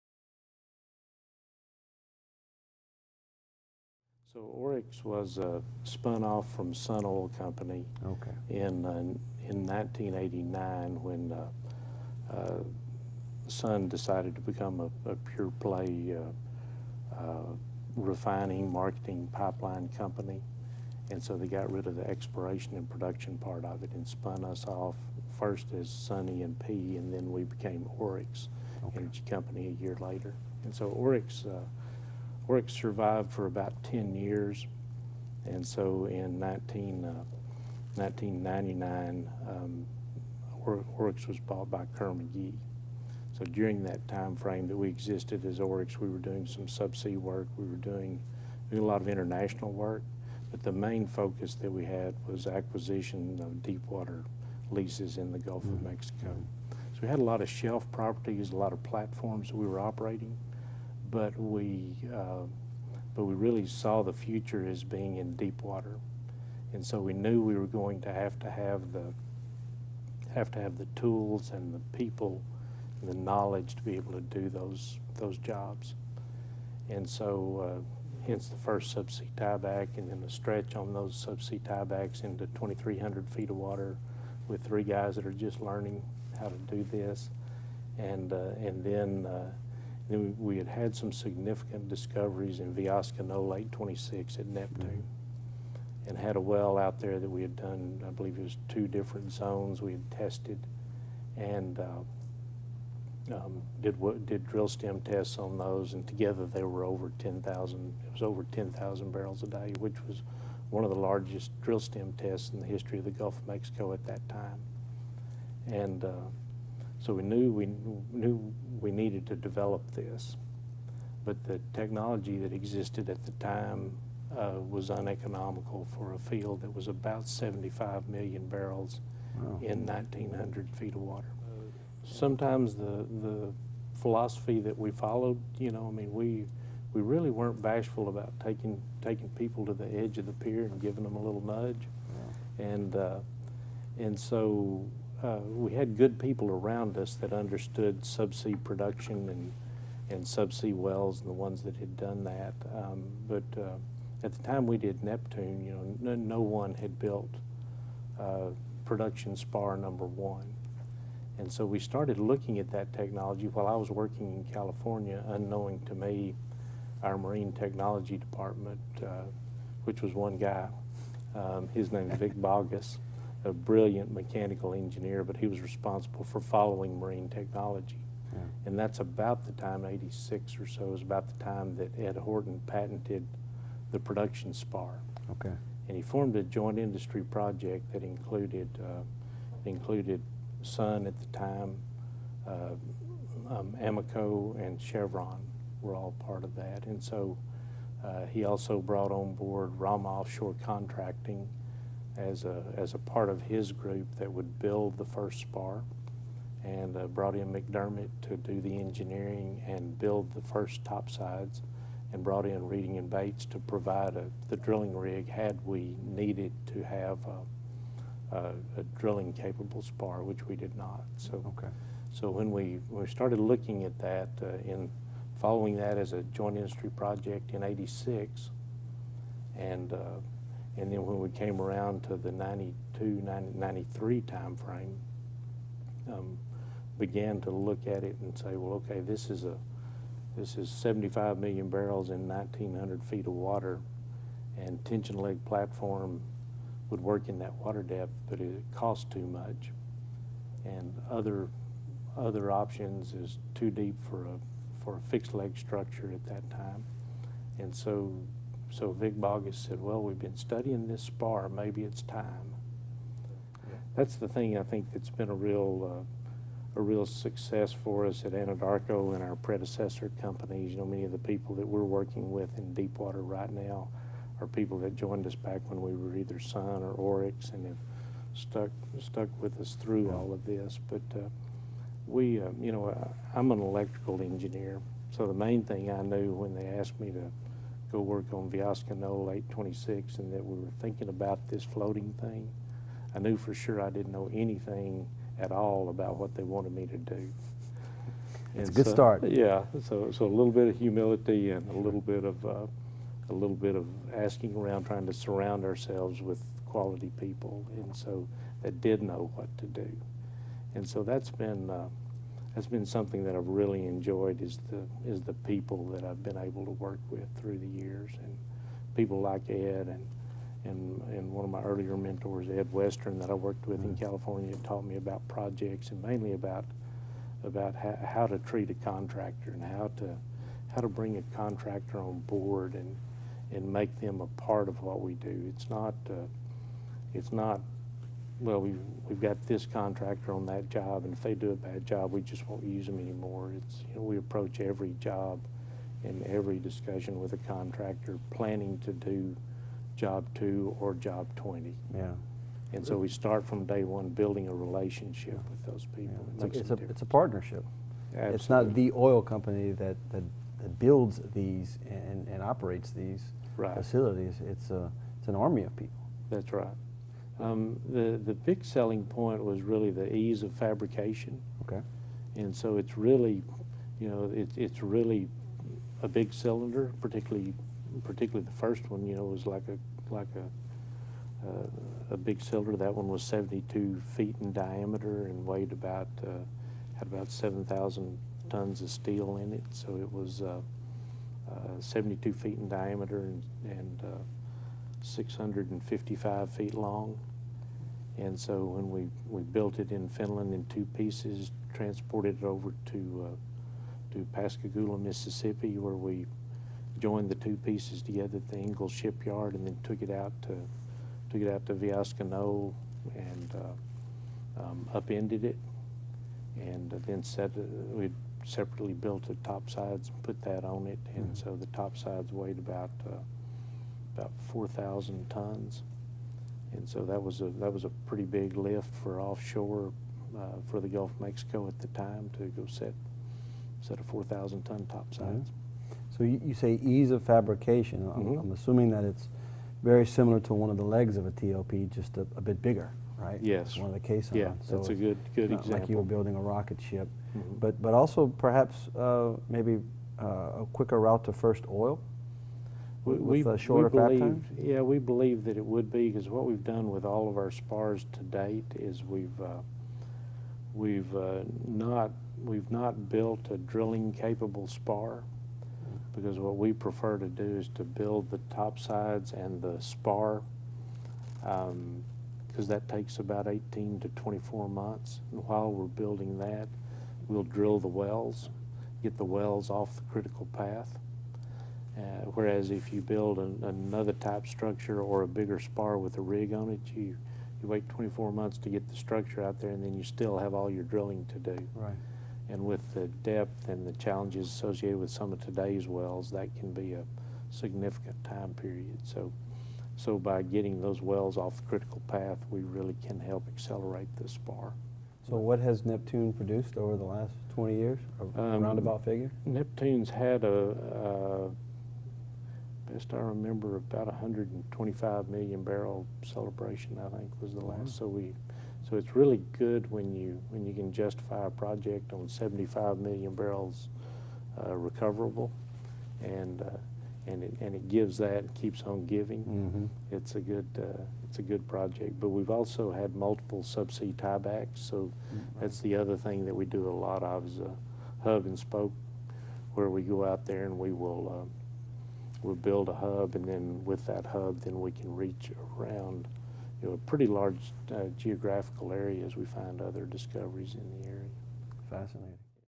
Vardeman, Don 2015
Don Vardeman championed successive applications of spar technology for deepwater development, and by his leadership in deepwater mega projects has provided an important example for Project Management in the offshore oil & gas industry. His untiring efforts in technology, academic and business societies, as well as his willingness to mentor colleagues and other young professionals have advanced the entire industry.

Vardeman has 40 years of experience in production engineering and operations. Graduating from Texas A&M University in 1975 with a B.S. in Electrical Engineering, he began his career with leadership roles in the development of deepwater fields utilizing spar technology that was first applied at the Neptune field. Vardeman led the project management teams for deepwater developments including the Nansen, Boomvang, Gunnison, Red Hawk and Constitution spars, as well as the Independence Hub semi-submersible production facility. Vardeman’s teams focused on the further use of spar technology, using teamwork, trusting relationships, and the transfer of learnings and knowledge from past to future projects. Vardeman is responsible for eight (8) spar developments and three (3) successive generations of spar technology, while helping to develop tomorrow’s deepwater leaders.

In November 2005, Vardeman was named Commander of the Order of the Lion of Finland by the President of Finland for meritorious service to the maritime industry of Finland, where most spar hulls have been built. Currently, he serves on the Engineering Advisory Council at Texas A&M University and formerly served as the Chairman of the Offshore Technology Conference.

Interviewed and edited by historian Jason Theriot.